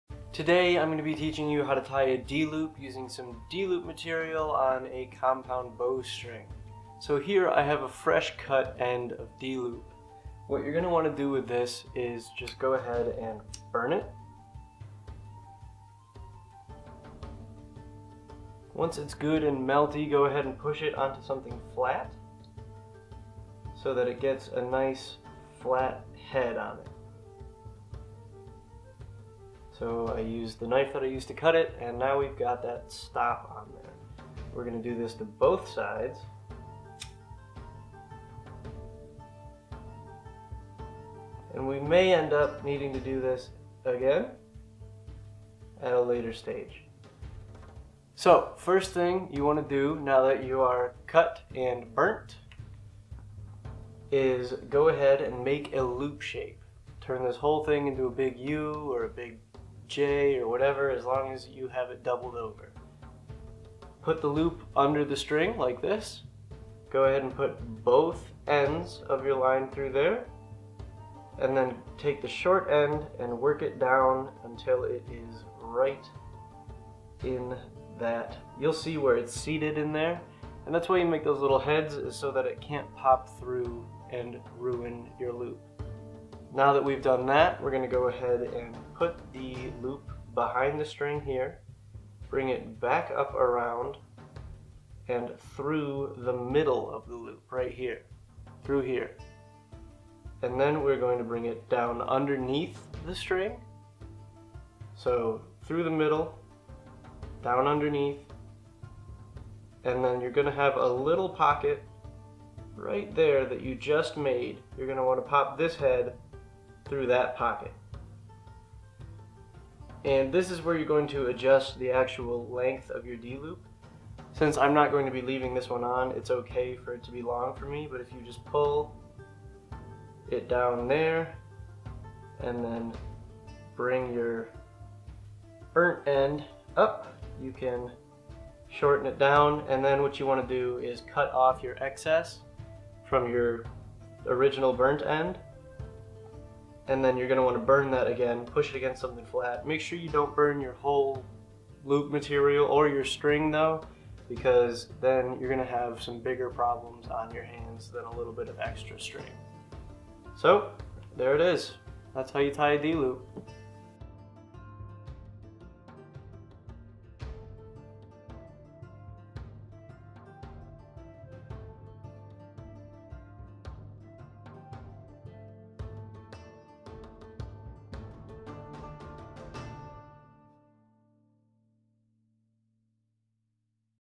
How to Tie a D-Loop - Easy to See!
In this video, I teach you how to tie a d-loop for target archery, hunting, 3D archery, or any other way you may be using your bow. Learning to tie a D-Loop is a very important skill for every archer to have, from beginner to expert!